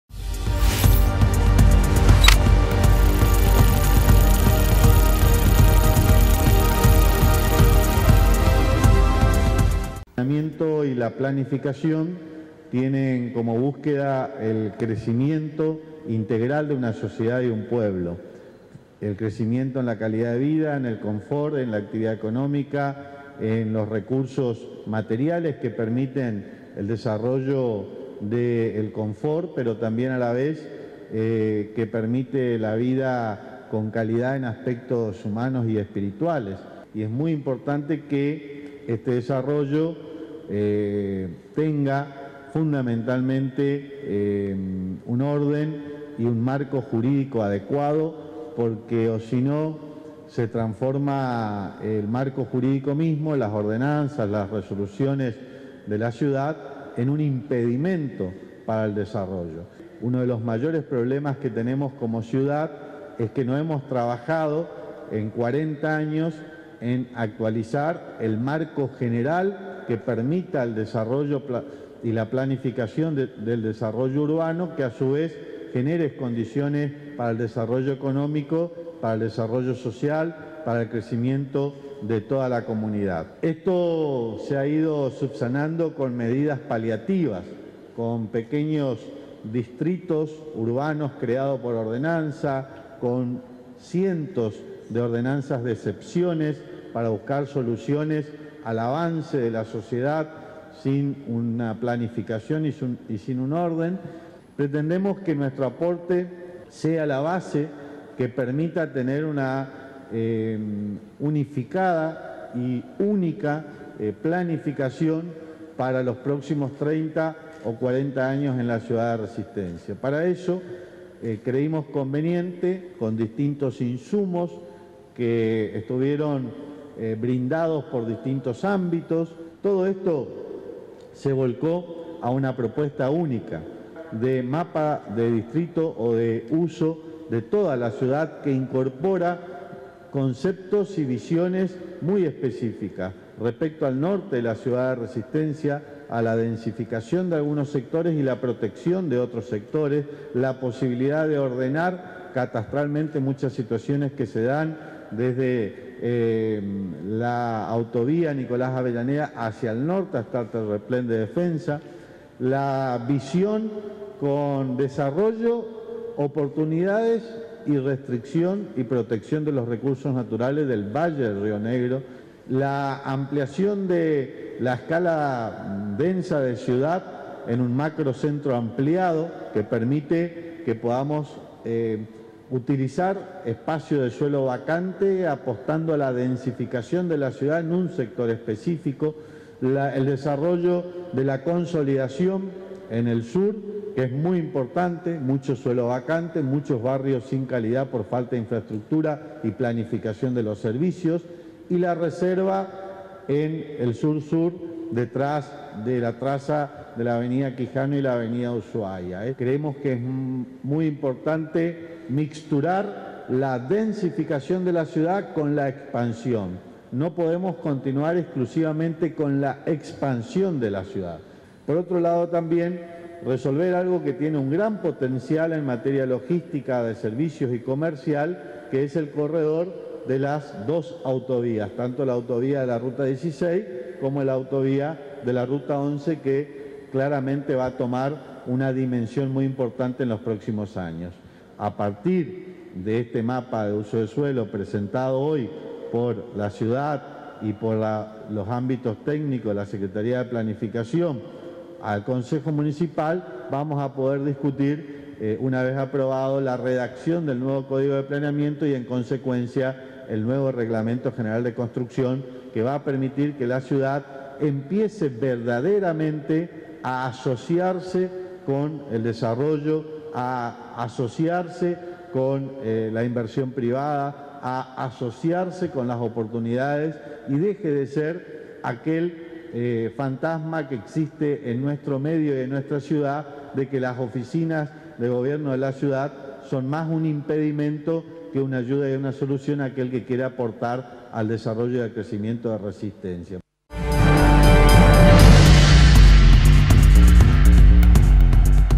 N9 FEDERAL - MARTINEZ - CHACO - RESISTENCIA AVANZA CON EL PLAN DE CRECIMIENTO SOSTENIBLE
PRESENTARON EL NUEVO MAPA DE USOS Y DISTRITOS PARA LA CIUDAD
Con el objetivo de continuar trabajando para lograr una Ciudad con desarrollo sostenible y planificado, Gustavo presentó el proyecto de un nuevo mapa de usos de suelo y distritos que propone estrategias para hacer frente a las demandas y requerimientos en la medida en que la población aumente. La propuesta, que será presentada ante el Concejo Municipal para su aprobación, divide a Resistencia en seis zonas diferenciadas geográficamente: zona norte, zona de las autovías de las rutas nacionales 11 y 16, zona del valle del río Negro, zona macrocentro ampliada, zona sur y zona de la reserva Ex Campo de Tiro.
NOTA: GUSTAVO MARTINEZ - INTENDENTE DE RESISTENCIA